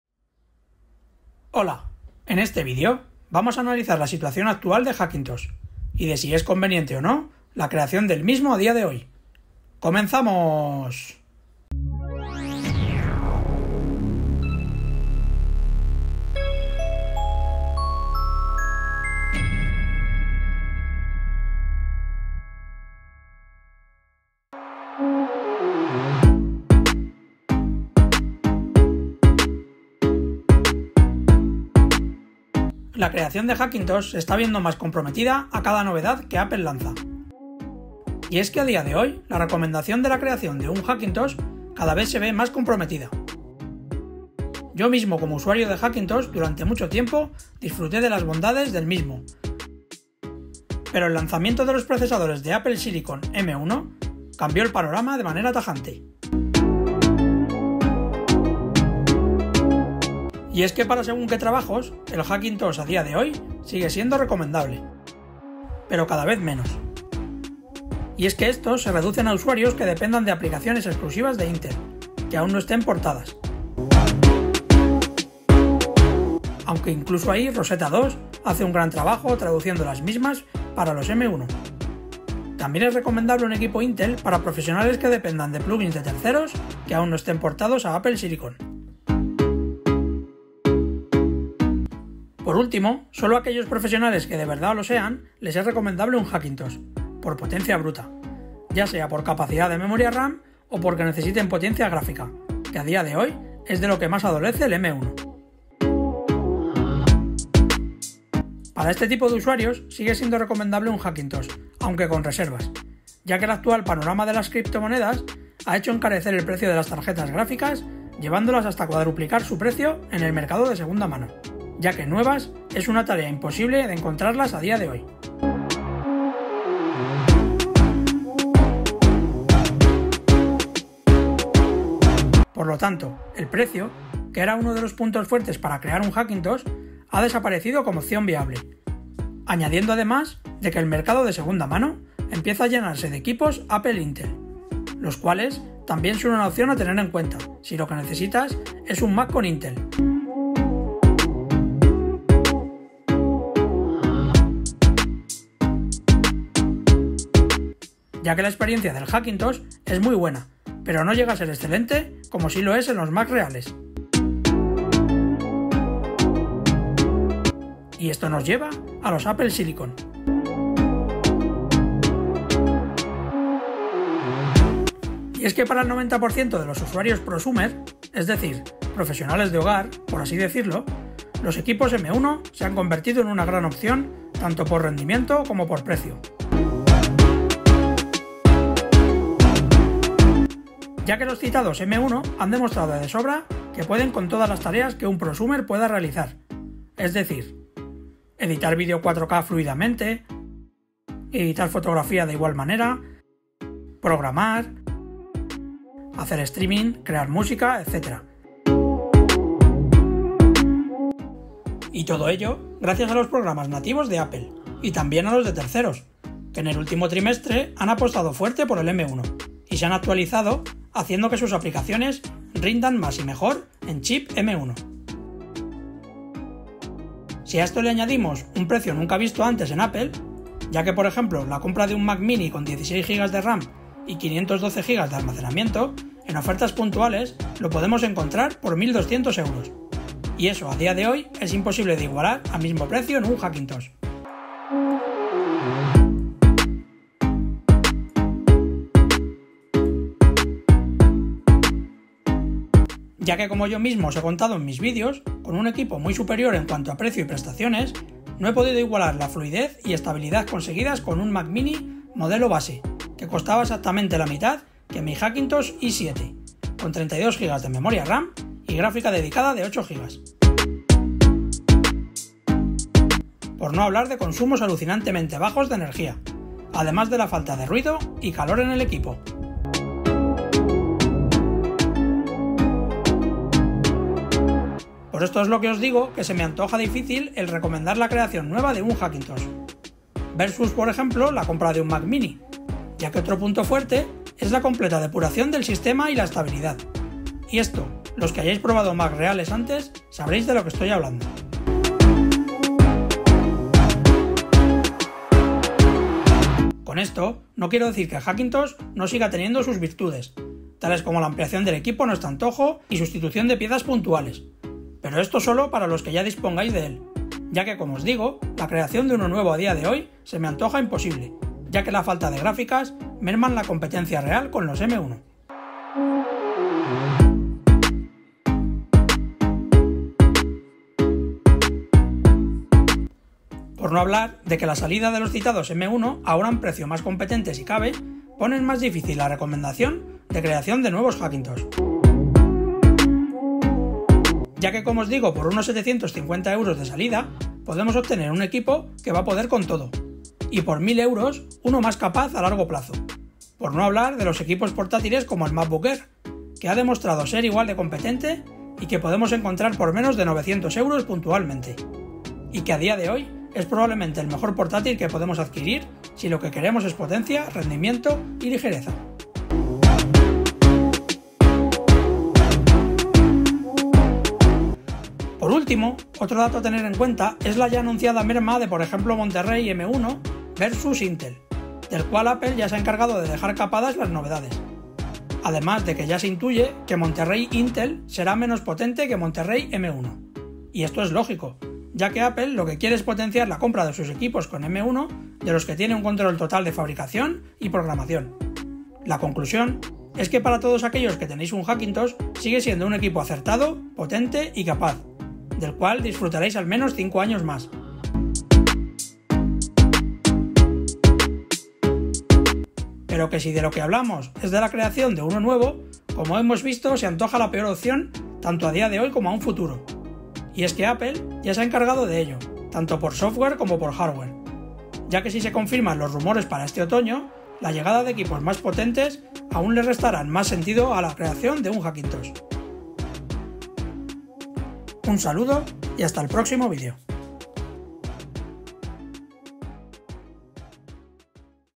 Hackintosh, es recomendable en 2021??
Analizamos la situación actual del panorama Hackintosh , y de si es recomendable o no la creación de uno en este momento y si es viable a futuro.

Enlace de Mx Master

Mac mini
adaptador nvme
adaptador thunderbolt
adaptador puertos
base satechi
base fideco